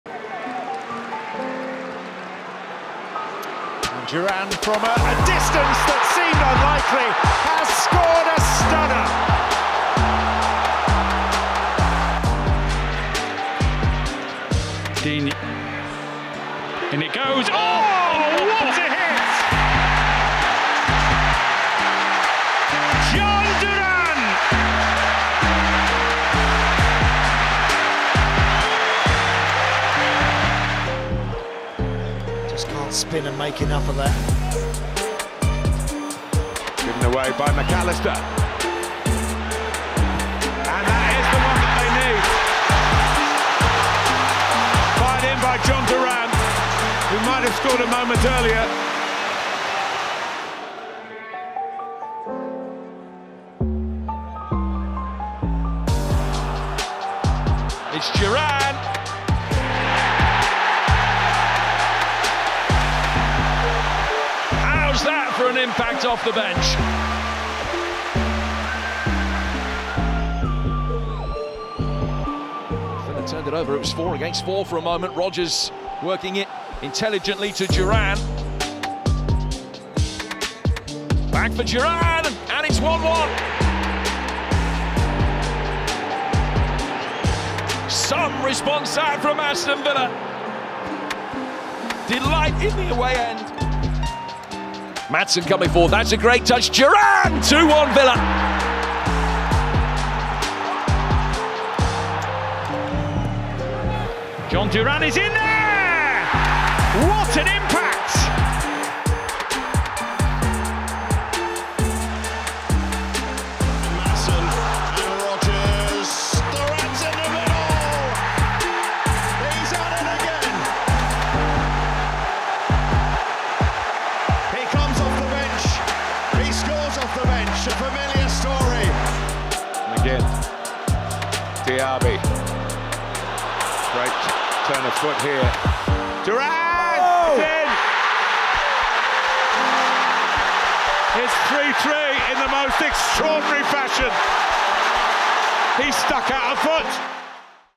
EVERY Jhon Duran goal in the Premier League! 💥 | Colombian star signs new Aston Villa contract ✍
Watch every goal by Jhon Duran goal in the Premier League so far, after the Columbian forward penned a new long-term Aston Villa deal.